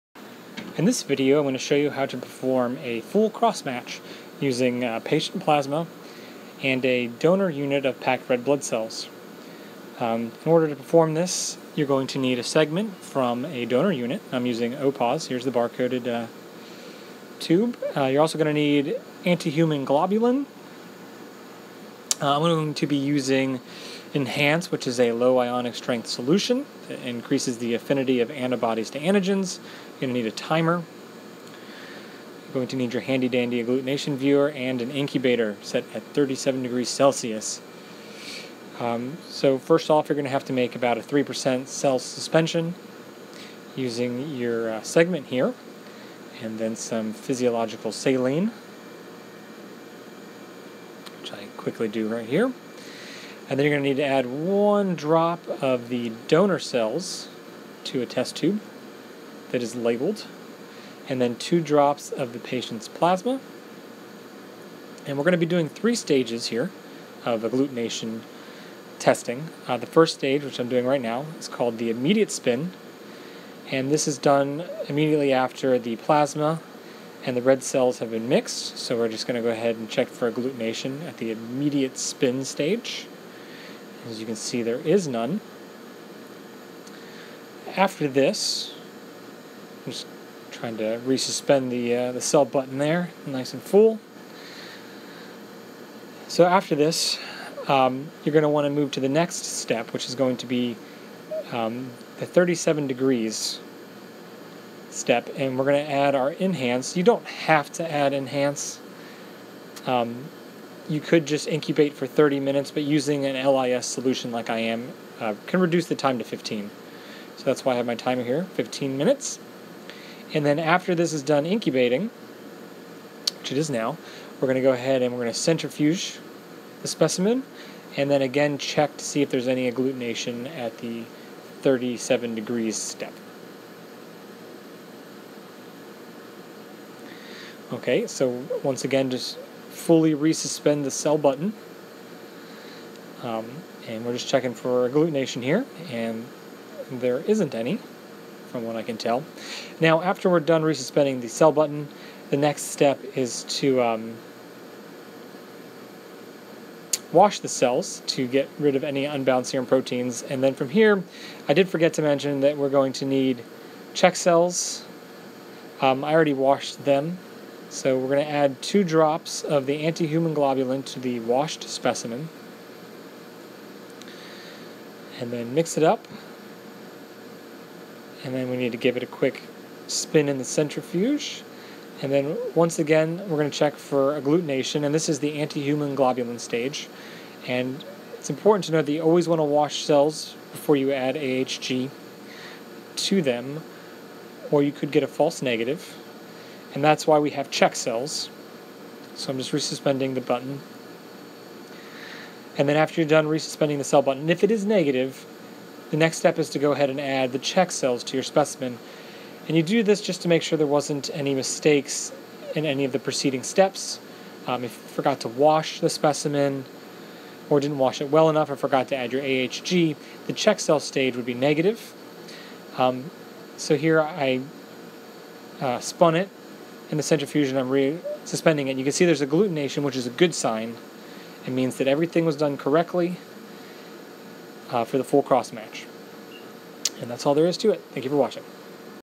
Blood Bank- Full Crossmatch (XM)
In this video I show you how to preform a full Crossmatch using a Low ionic strength solution (LISS). In the video I check for agglutination at 3 stages: immediate spin, 37 degrees Celsius, and AHG. Finally, I check my work using check cells.